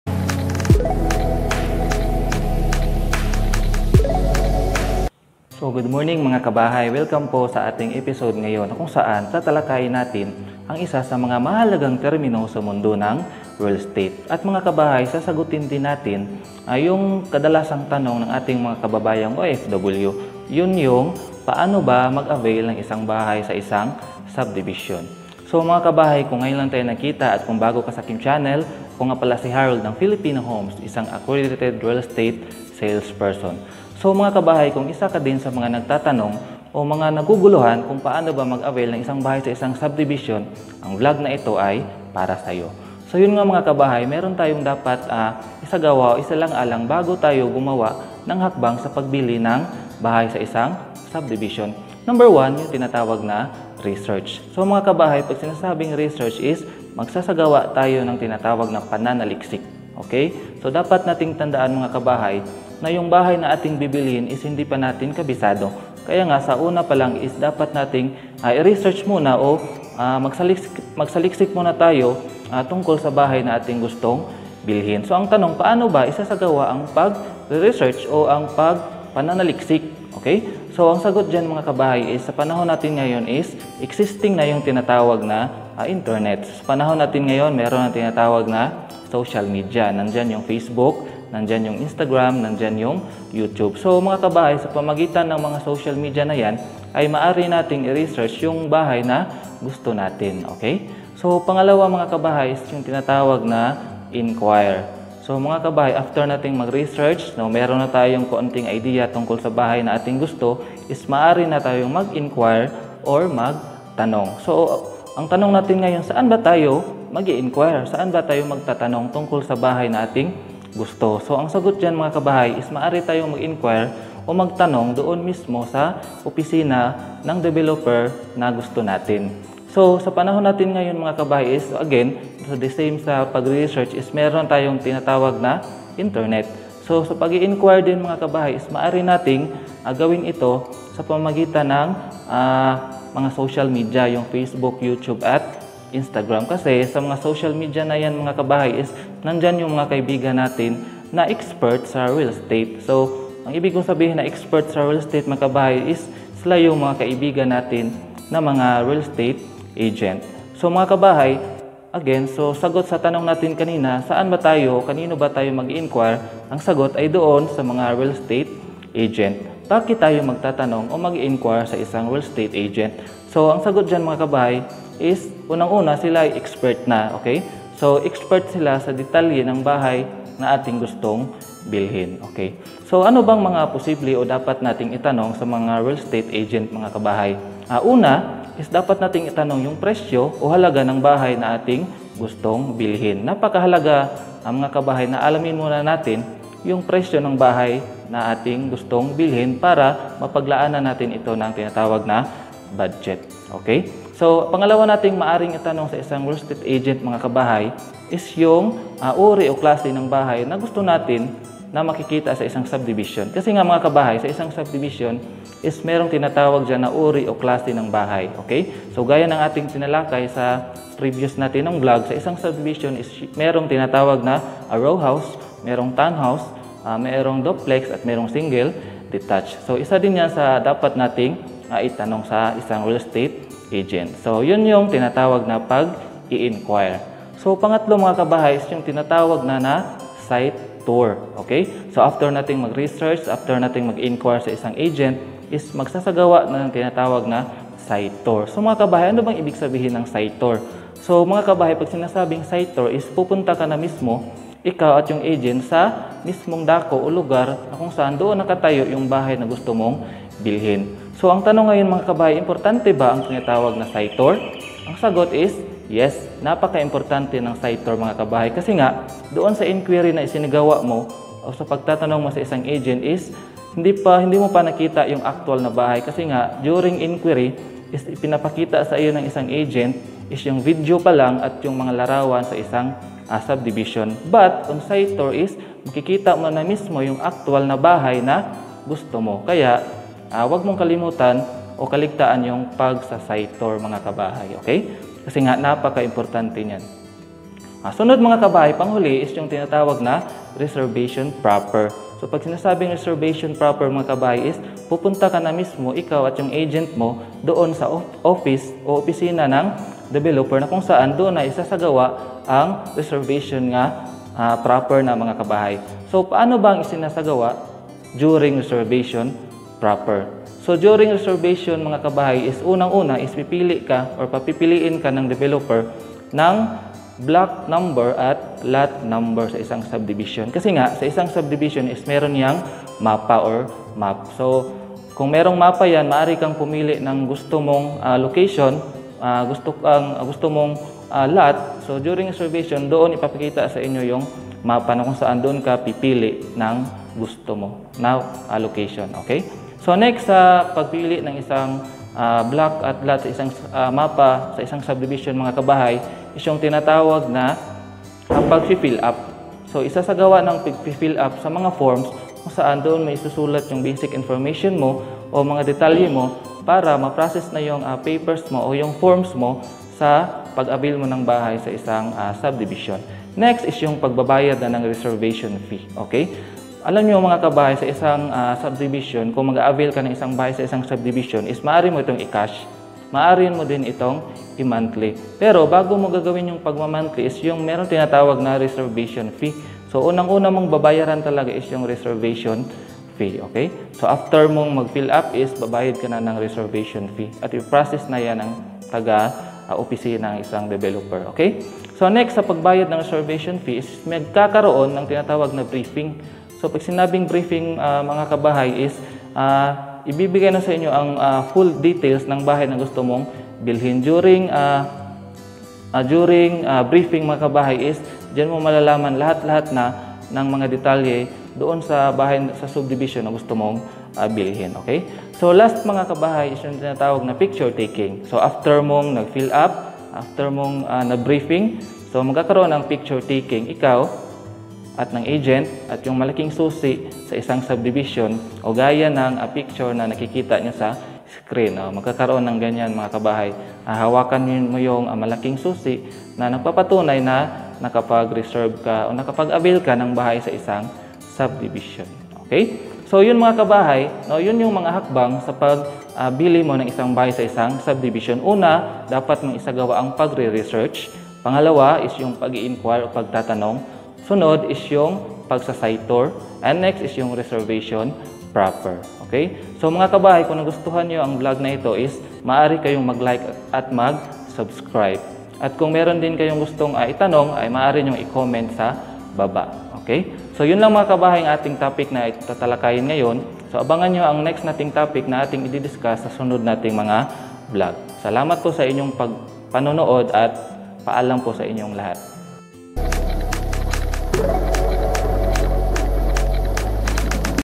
USAPANG BAHAY # 4 - PAANO MAG AVAIL NG BAHAY SA ISANG SUBDIVISION NA WALANG HASSLE?
Kung isa ka sa mga nagtatanong at naguguluhan na kong papaano ba talaga makakabili ng bahay sa isang subdivision na walang hassle ay ang vlog na ito ay para sayo. Enjoy!